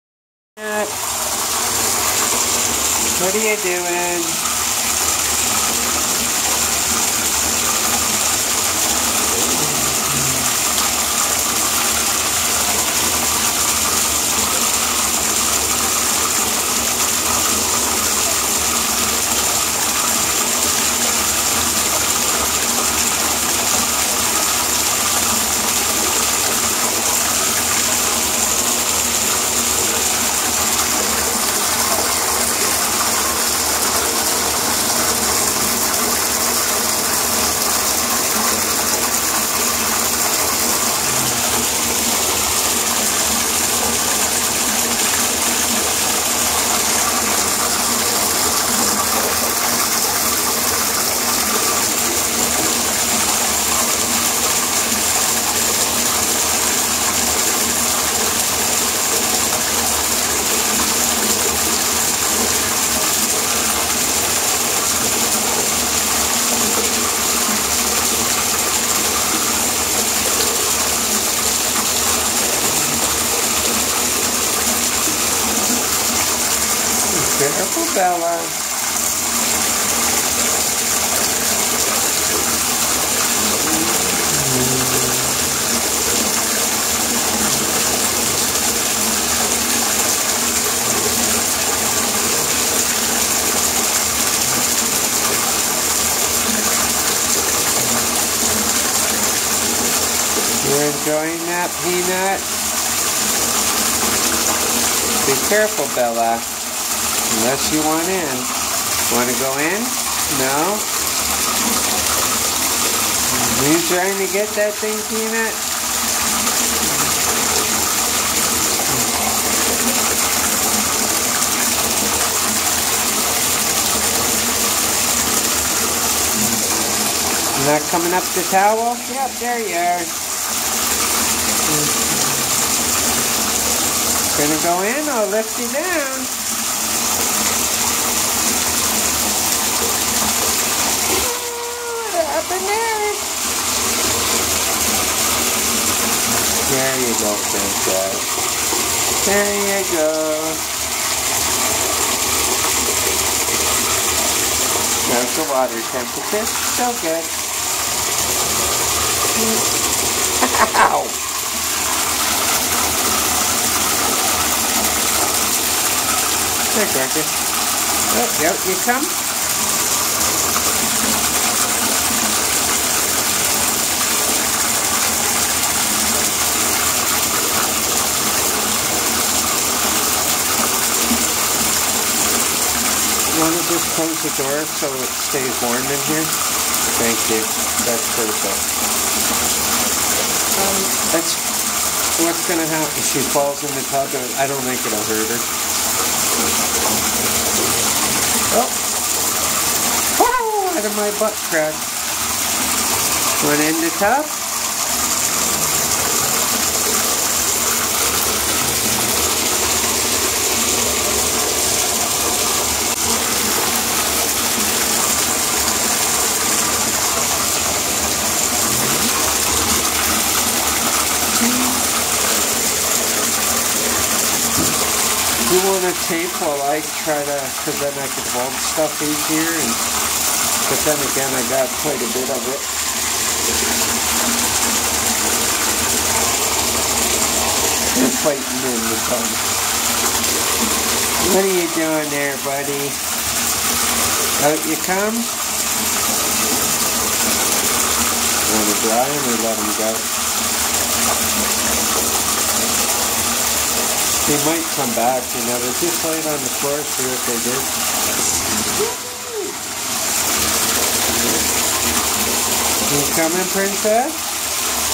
FERRET BATH TIME
Peanut and Bella play in the tub with water running. Stewart Luna and Willy dont like the water. Hope onw day the will